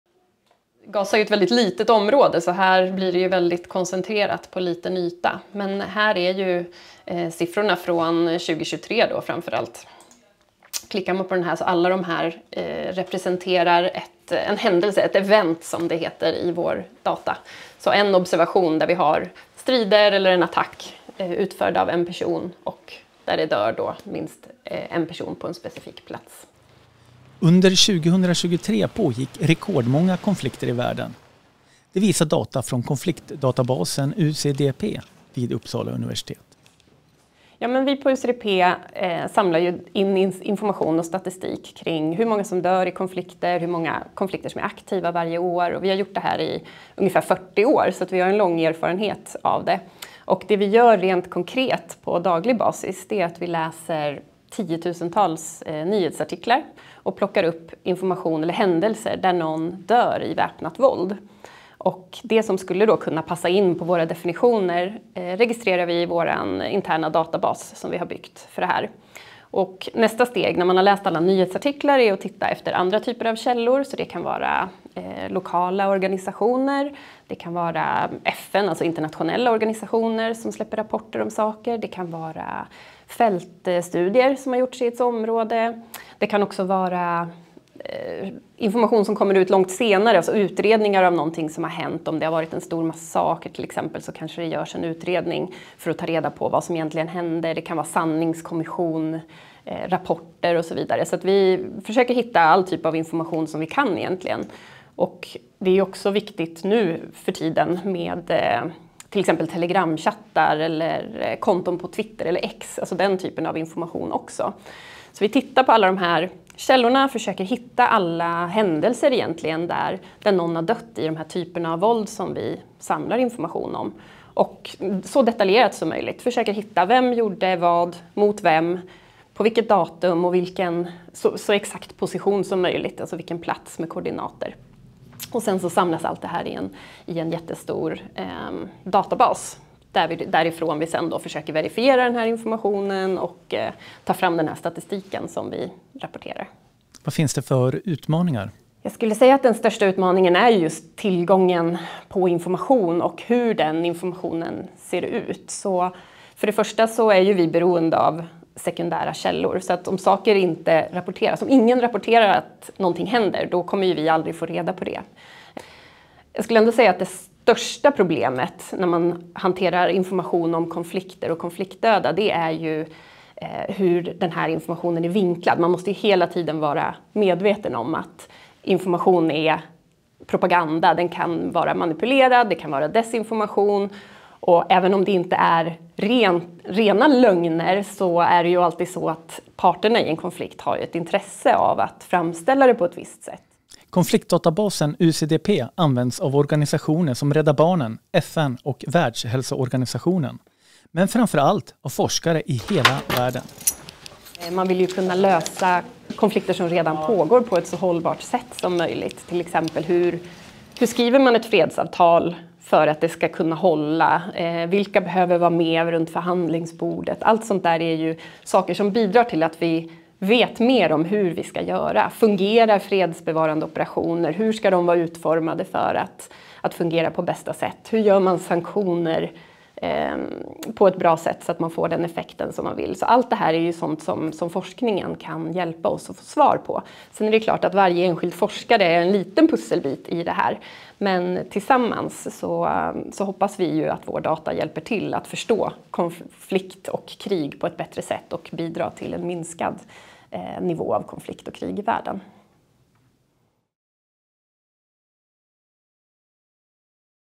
De samlar data om världens konflikter
Under 2023 pågick rekordmånga konflikter i världen. Det visar data från Konfliktdatabasen UCDP vid Uppsala universitet. Forskarna på UCDP samlar in information och statistik kring hur många som dör i konflikter och hur många konflikter som är aktiva varje år. Intervju med Therese Pettersson, forskningssamordnare vid Institutionen för freds- och konfliktforskning, Uppsala universitet.